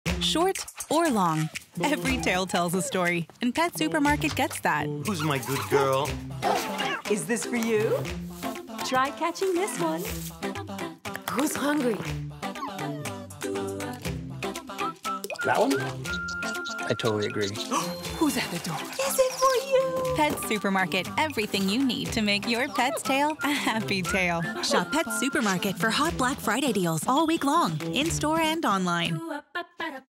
Pet Supermarket Black Friday Week 2023
Indulge your furry friends with a week-long extravaganza of unbeatable deals on all things pet-related. Save BIG, in-store and online, all Black Friday week long! Visit petsupermarket.com for details